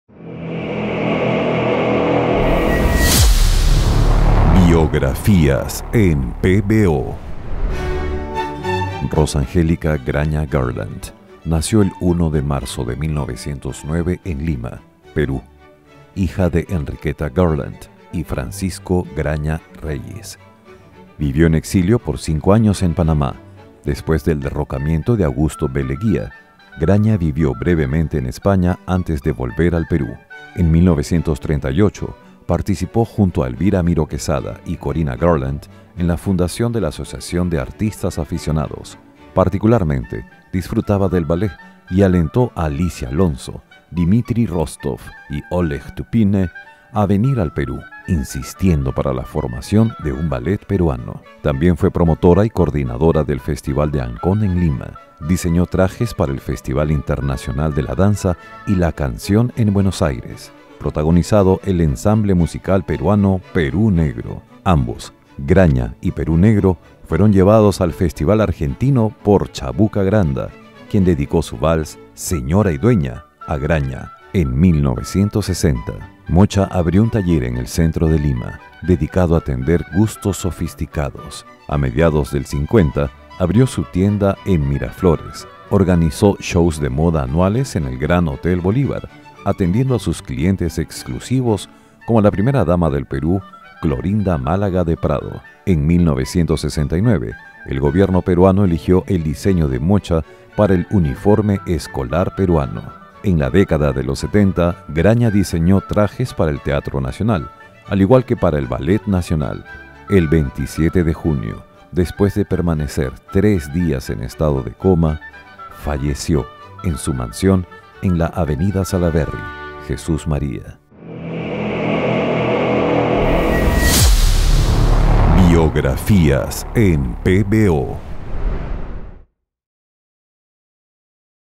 Todo lo que debes saber sobre MOCHA GRAÑA en BIOGRAFÍAS PBO 🗣️
BIOGRAFÍAS PBO 🗣️
👉 Mocha Graña

🗣️ ¿Qué biografía te gustaría aprender?
📻 91.9FM ¡LA RADIO CON FE!